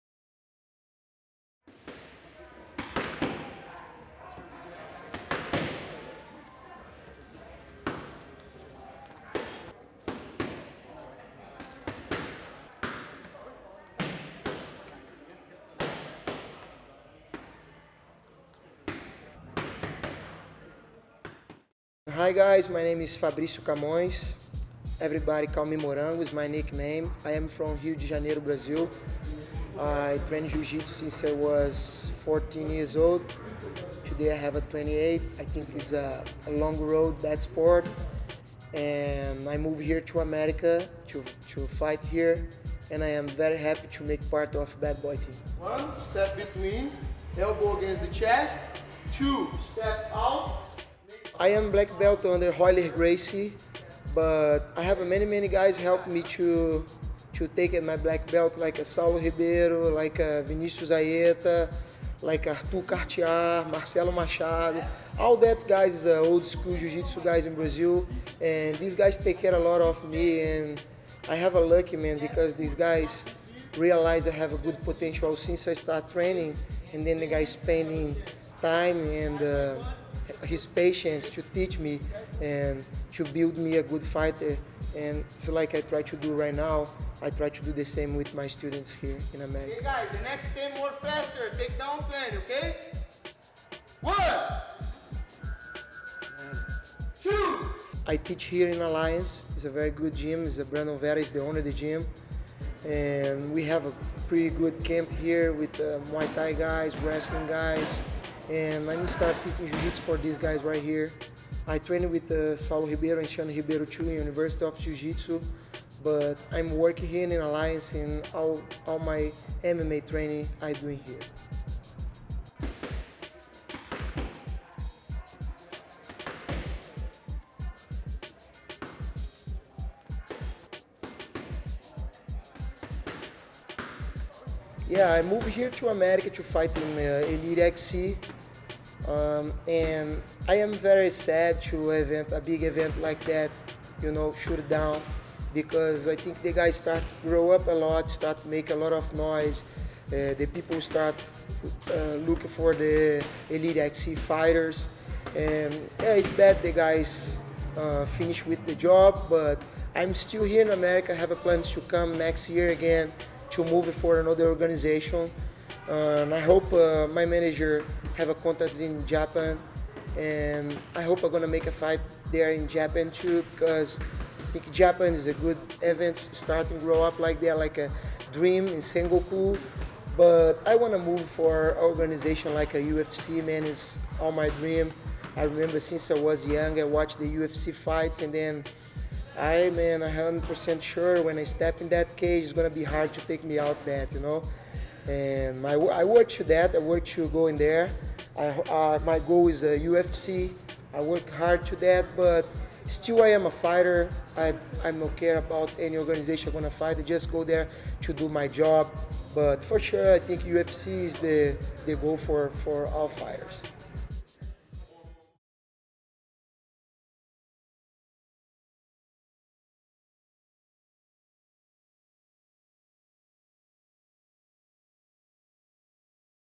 Bad Boy Fighter Fabricio "Morango" Camoes
Watch Royler Gracie Black Belt and Elite XC Lightweight Contender
Fabricio "Morango" Camoes training at Brandon Vera's gym Alliance MMA in Chula Vista, CA.